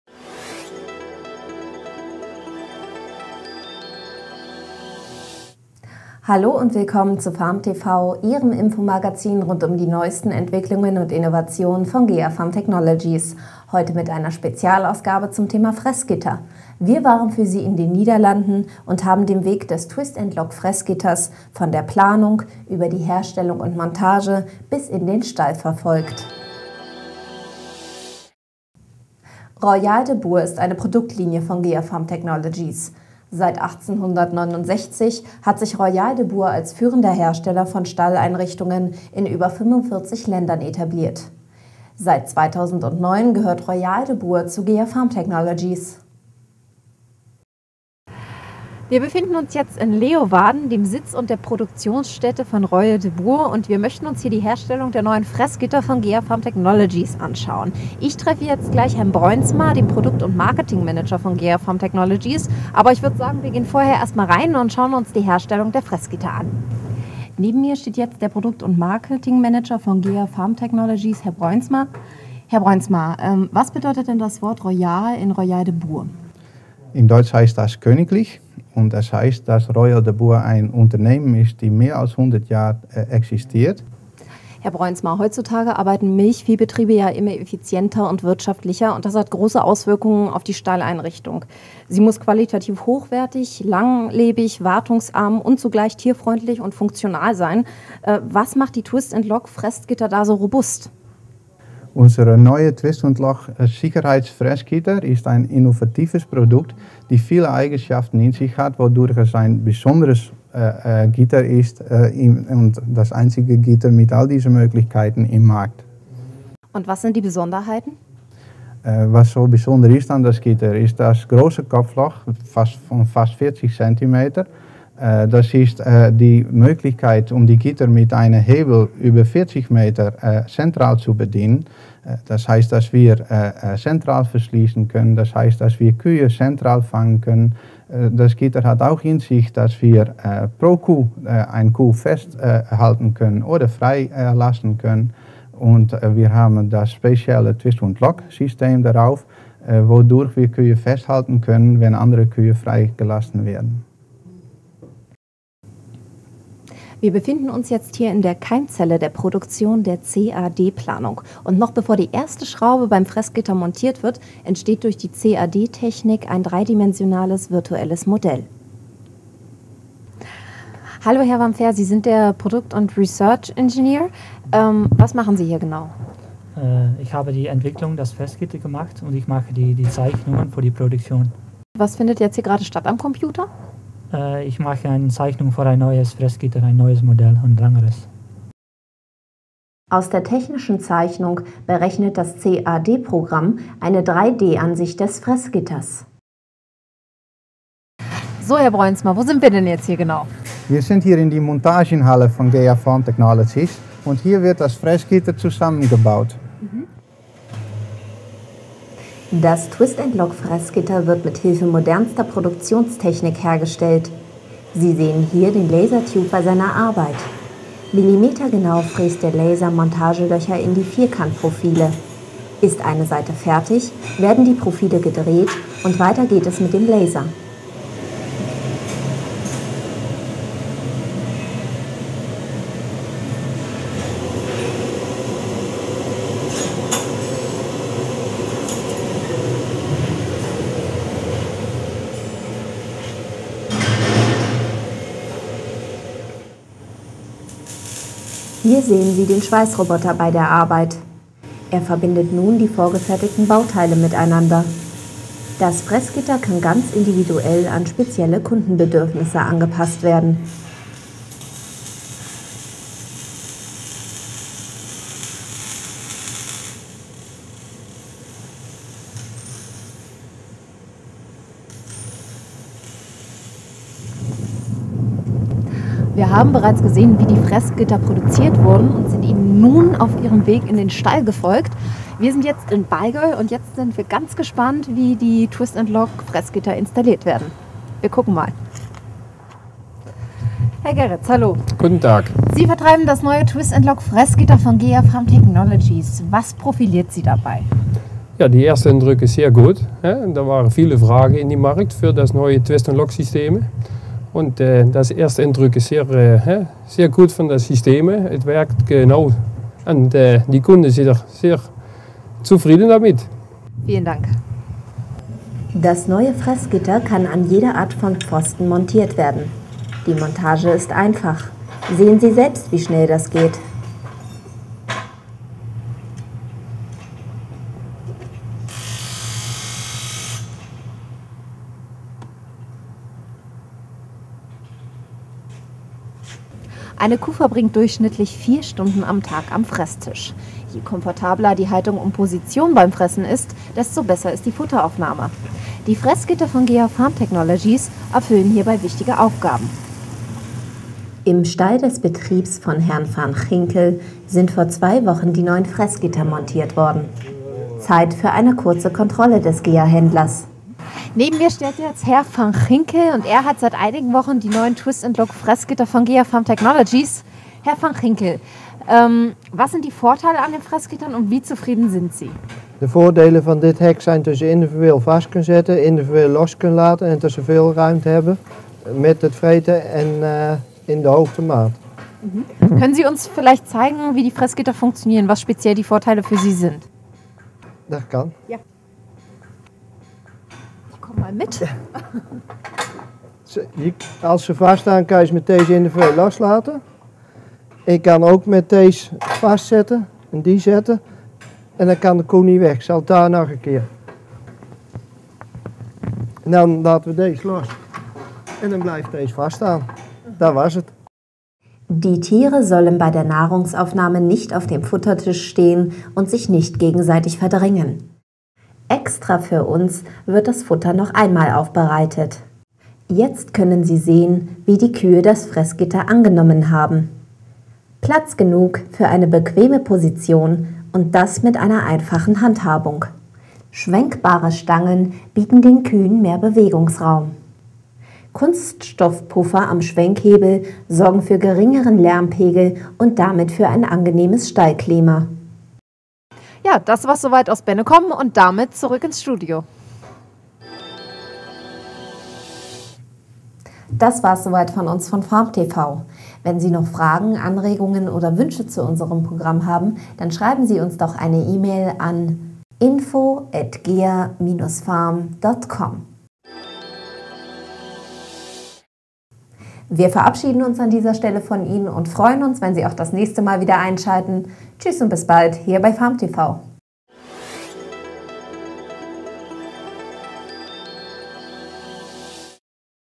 GEA Farm TV Oktober 2011 - Spezial: Neue Fressgitter
Spezial: Neues Fressgitter - von der Produktion bis in den Stall